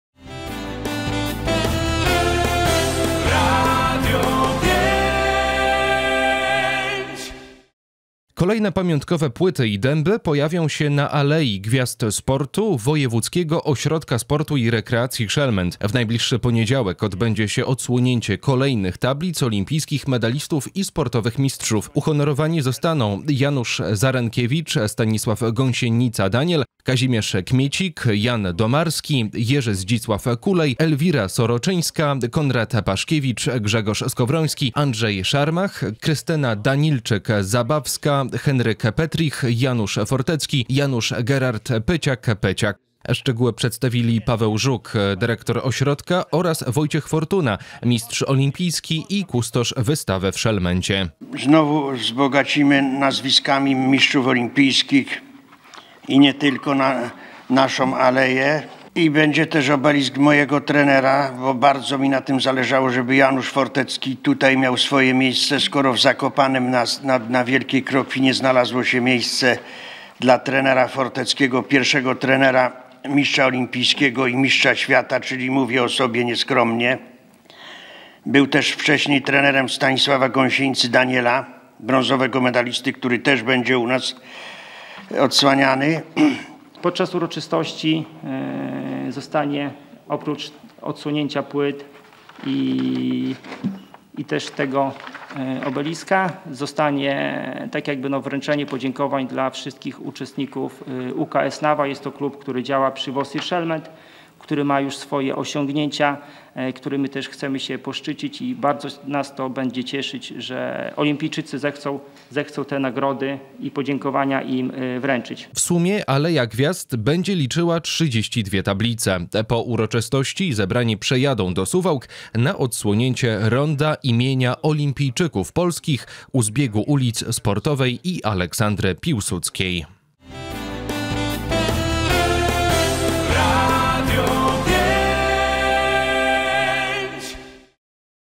Na Szelmencie przybędzie gwiazd
Kolejne pamiątkowe płyty i dęby pojawią się na Alei Gwiazd Sportu Wojewódzkiego Ośrodka Sportu i Rekreacji Szelment.
 W najbliższy poniedziałek (14.09) odbędzie się odsłonięcie kolejnych tablic olimpijskich medalistów i sportowych mistrzów.
  Uhonorowani zostaną: Janusz Zarenkiewicz, Stanisław gąsienica Daniel, Kazimierz Kmiecik, Jan Domarski, Jerzy Zdzisław Kulej, Elwira Seroczyńska, Konrad Paszkiewicz, Grzegorz Skowroński, Andrzej Szarmach, Krystyna Danilczyk - Zabawska,  Henryk Petrich, Janusz Fortecki, Janusz Gerard Pyciak-Peciak. Szczegóły przedstawili Paweł Żuk, dyrektor Ośrodka oraz Wojciech Fortuna, mistrsz olimpijski i kustosz wystawy w Szelmencie.